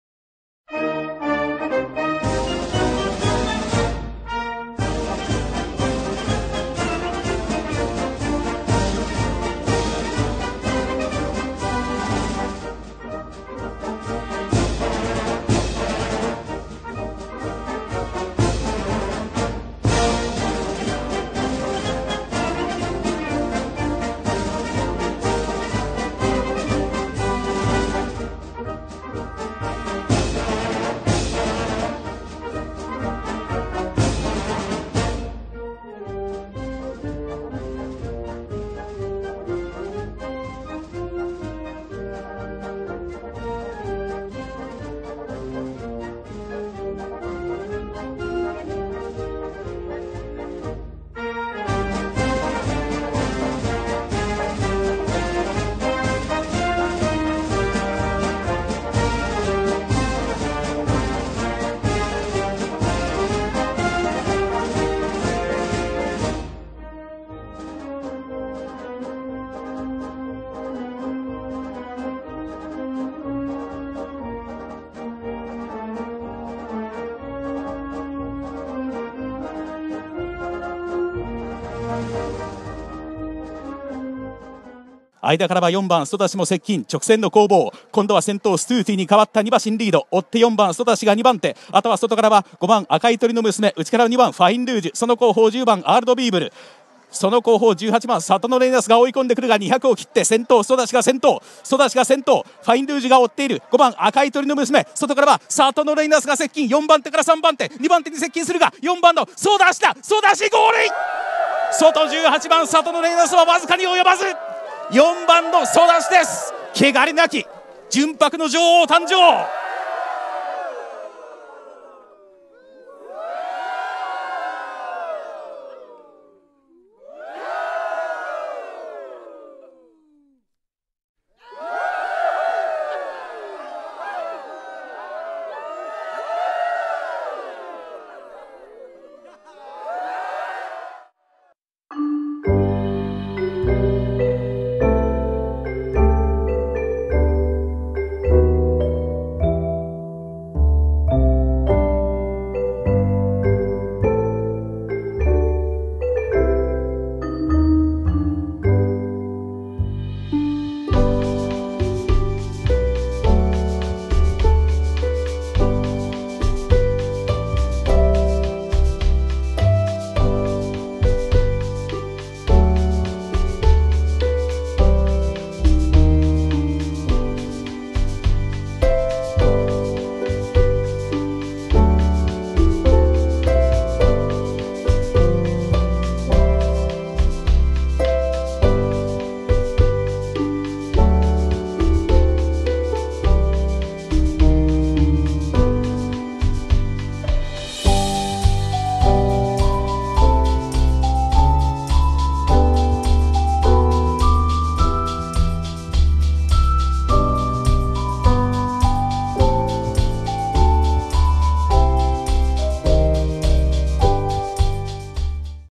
【競馬】泰河の勝負馬券 桜花賞２０２１ １０万円勝負編 【実践】３連複と３連単勝負
泰河、競馬　動画をご視聴頂きありがとうございます。
泰河の勝負馬券　G1編　３連複と３連単で勝負しました。